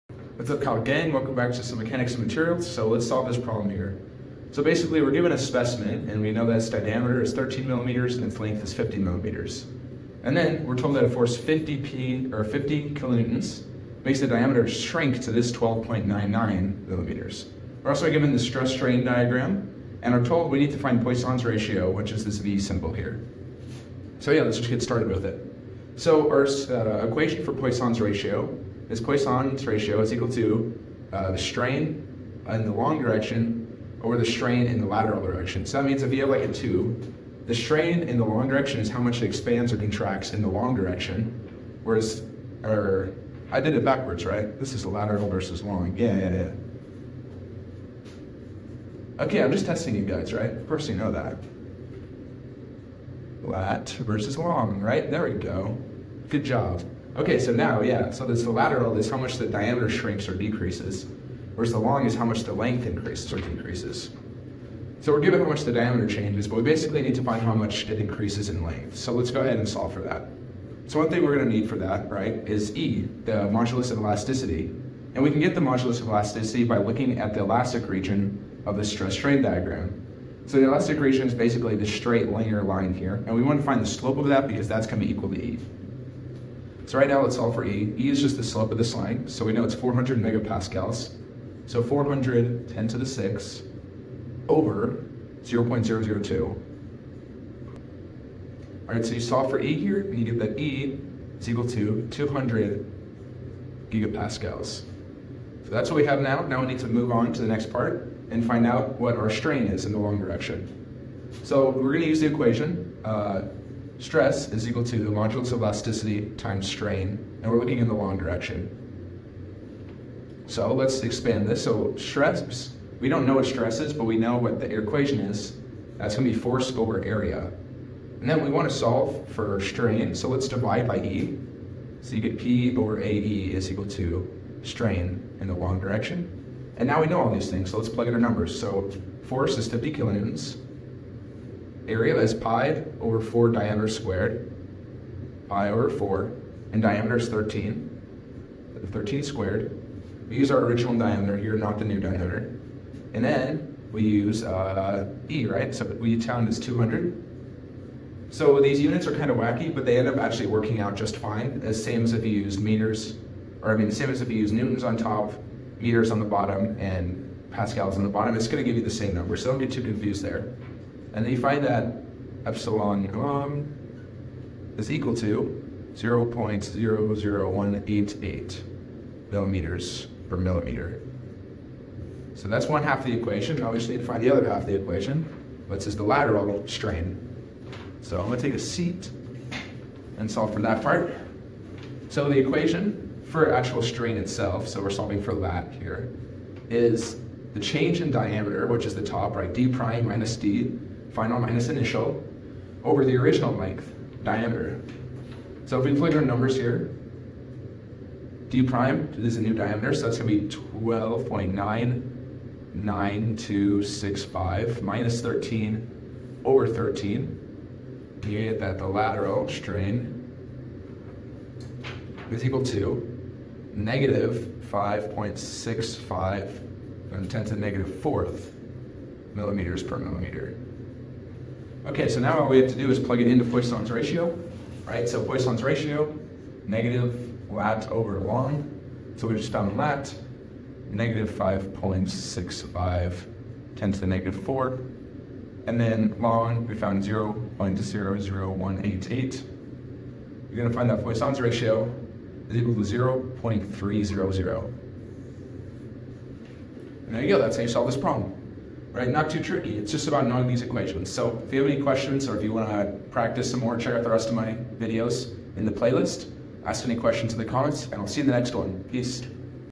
The elastic portion of the stress–strain diagram for a steel alloy is shown in the figure - 3-26
3–26. The elastic portion of the stress–strain diagram for a steel alloy is shown in the figure. The specimen from which it was obtained had an original diameter of 13 mm and a gage length of 50 mm. When the applied load on the specimen is 50 kN, the diameter is 12.99265 mm. Determine Poisson’s ratio for the material.

All problems from Mechanics of Materials Eleventh Edition by Russell C. Hibbeler.